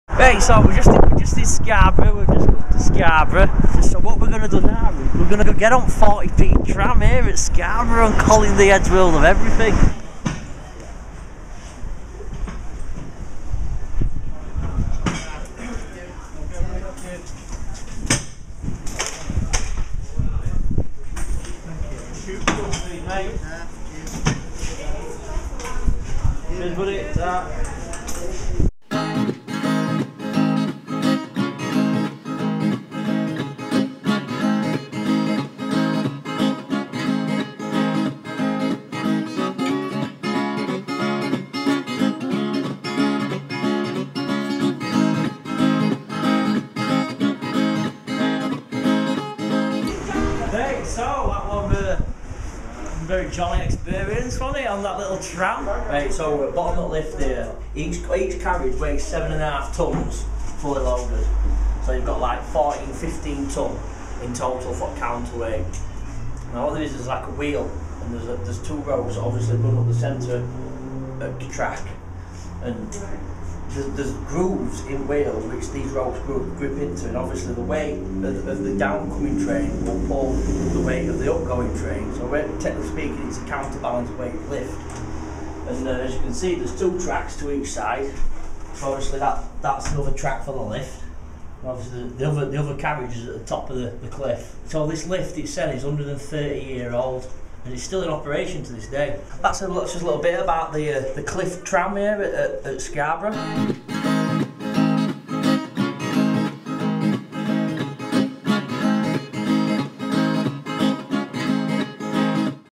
Colin rides The Cliff Tram in Scarborough
Scarboroughs oldest tourist attraction is surely this vintage counter weight lift tram which operates from a steep cliff offering access to Scarborough Bay